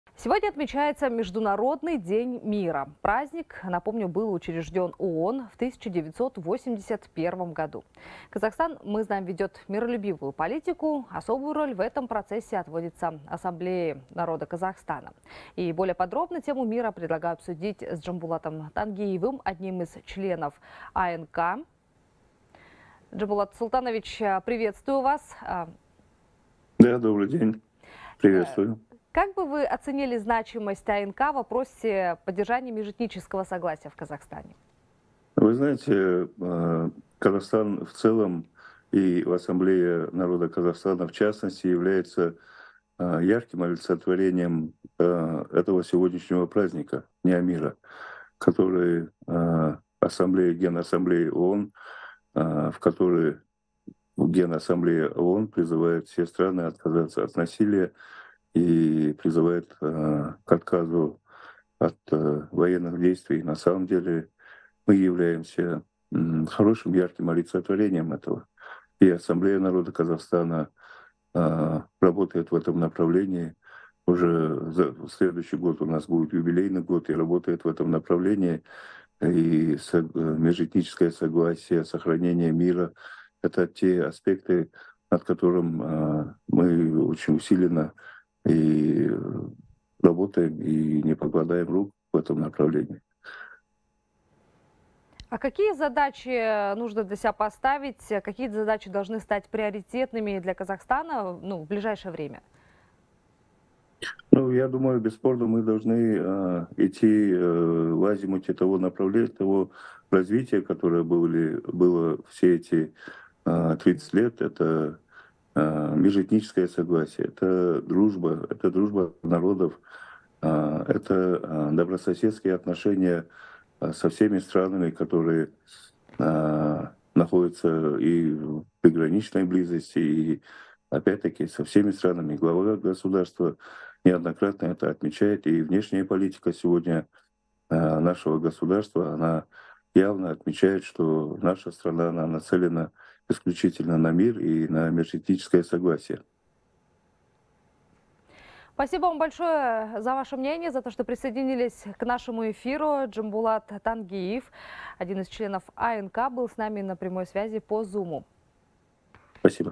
21 сентября – Международный день мира
Спикер:

Джамбулат Тангиев, член Ассамблеи народа Казахстана.